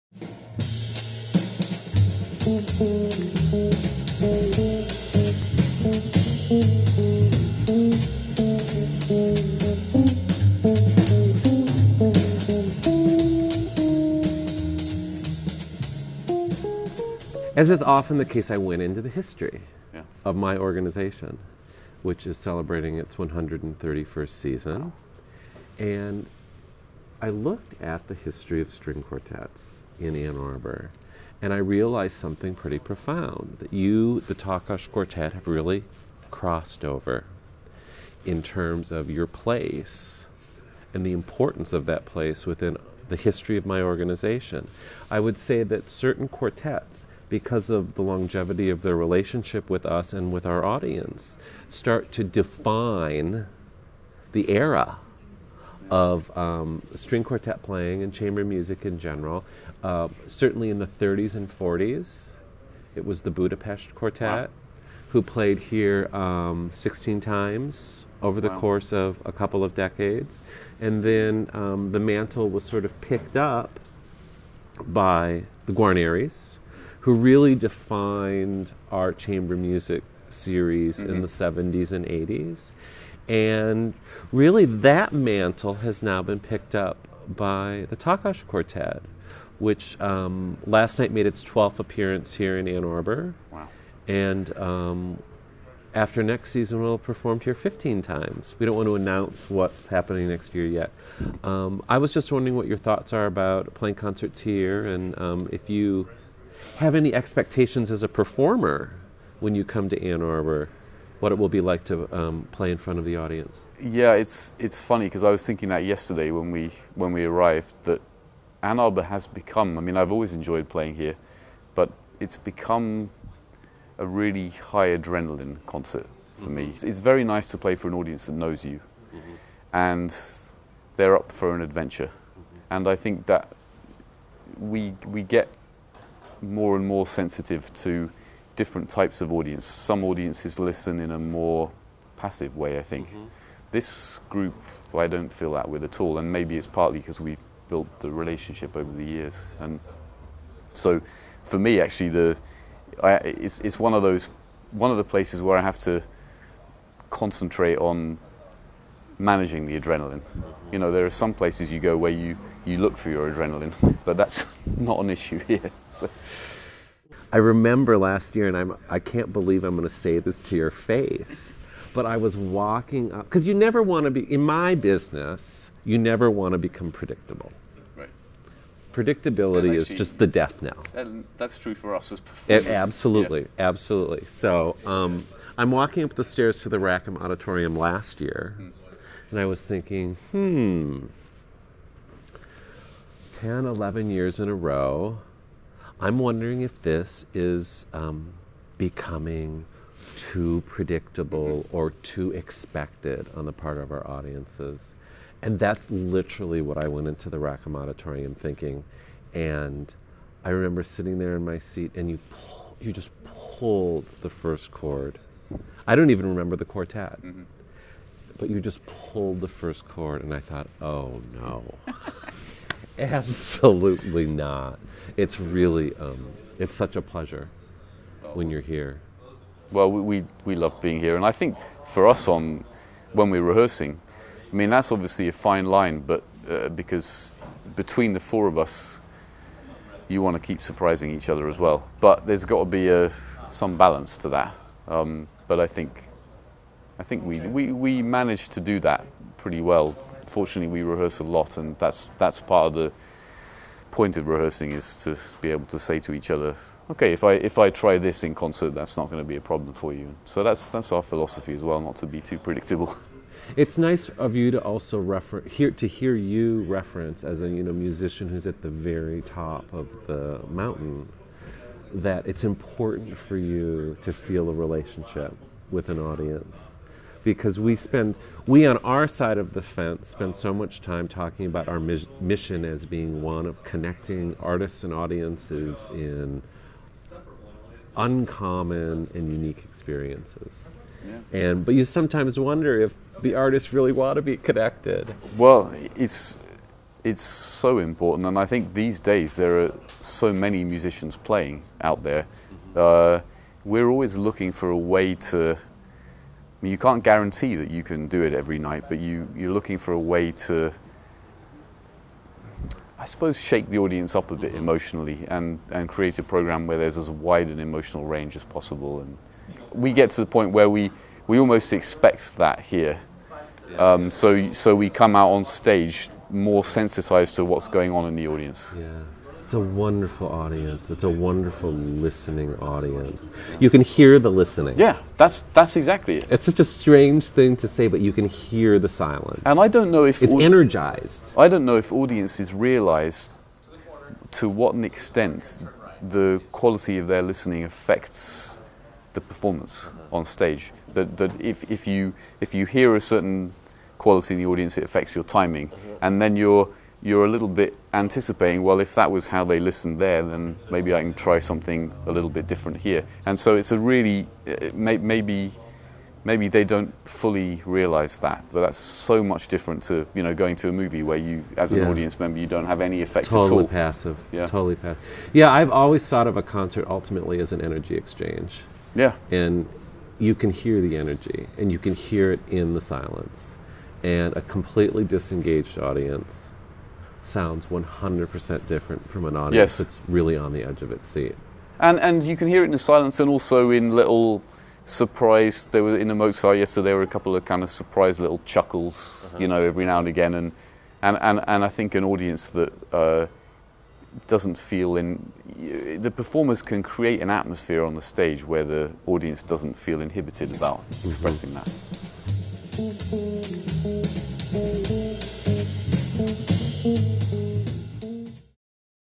UMS Interviews Edward Dusinberre, Takacs Quartet (Part 1)
In March 2010, UMS Director of Programming Michael Kondziolka had the chance to sit down with Edward Dusinberre, violinist of the Takács Quartet, to talk about what it's like to play for the Ann Arbor audience, why Ann Arbor audiences are special, and how the adrenaline always rushes before an Ann Arbor performance.